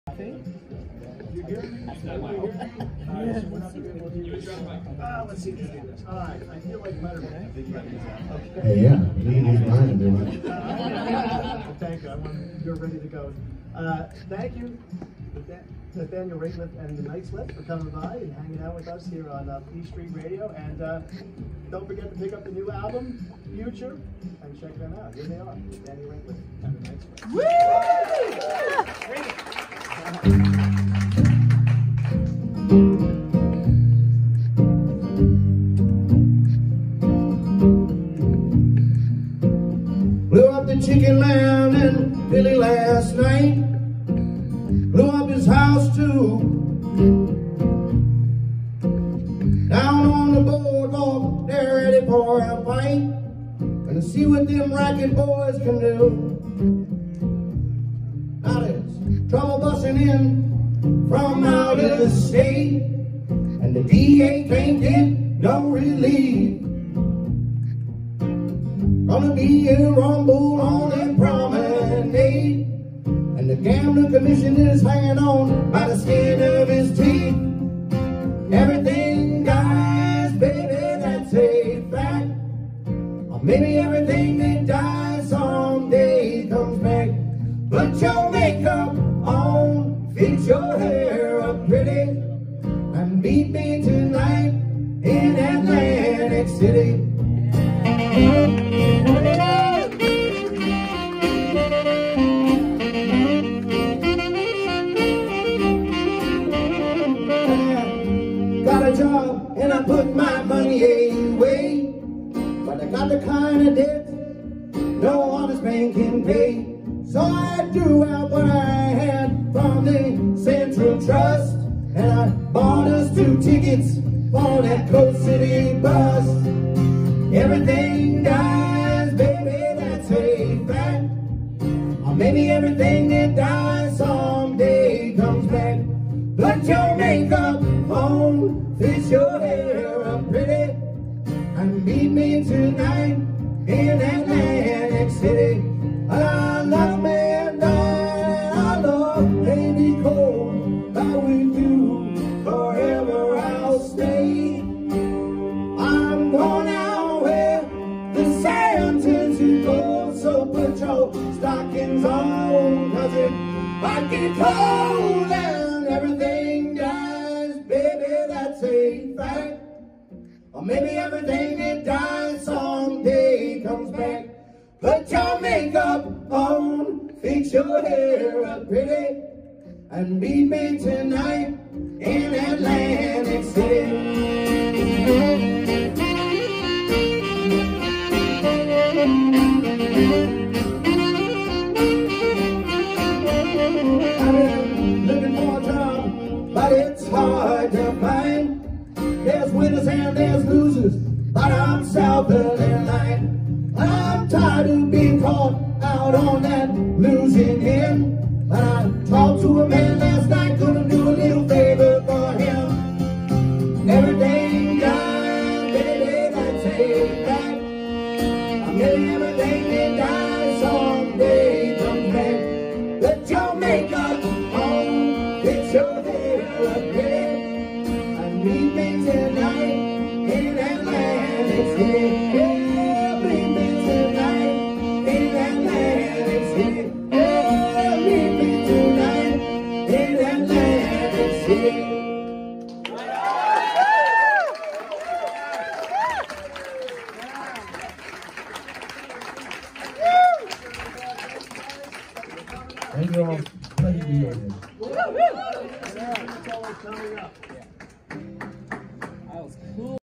Nathaniel Rateliff, Mark Shusterman, Jeff Dazey at Transparent Clinch Gallery ~ Rose A Montana Video
Appearing for The Wild & The Innocent with Jim Rotolo at International photographer Danny Clinch’s Transparent Gallery in Asbury Park on July 1, 2022 was Nathaniel Rateliff and two of his fabulous Night Sweats ~ keyboardist and harmonizer Mark Shusterman and saxophonist Jeff Dazey.